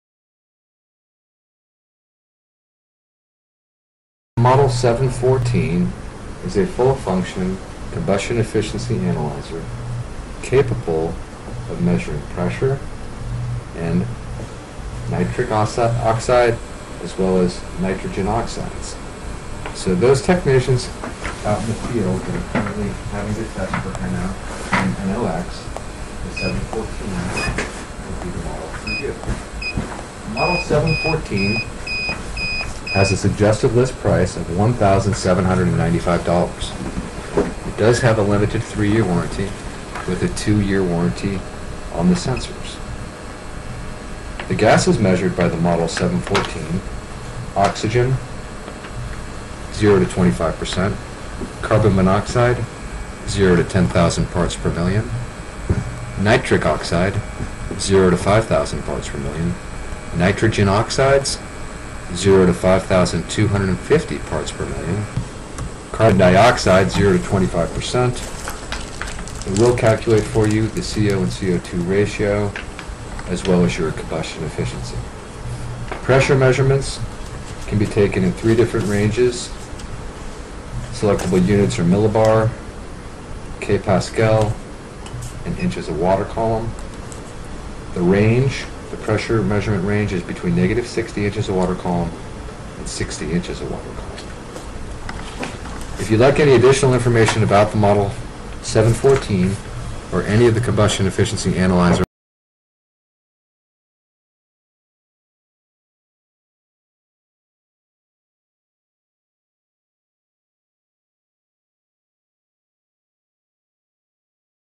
TPI 714 Combustion Gas Efficiency Analyzer
TPI 714 Combustion Gas Efficiency Analyzer Flue Gas CO2 CO NO NOx

The Test Products International 714 Combustion Gas Analyzer can test flue gases for oil and gas fired equipment such as condensing boilers and high-efficiency furnaces.

The 714 is a combustion gas efficiency analyzer which has an available (optional) PC interface. It's sensor for carbon monoxide features an on board filter that filters (nitric oxide) NO, (nitrogen dioxide) NO2 & (combination of NO2 and NO) NOX for cross sensitivity error prevention.

Enjoy quick and easy set ups, fast purge & the capability for performing fuel selections during startup which enables your tests to be carried out quickly with no extra set-up time. You may also use your most recently selected fuel for your default setting, which prevents you from performing a fuel selection when your analyzer is powered on.
Measure O2, CO & NOX
CO2, efficiency, excess air & NOX calculations
Determine combustion gas efficiency, CO air-free and CO2
Detection of combustible gas
PC communication (optional)
Data logging
NOX / NO measurements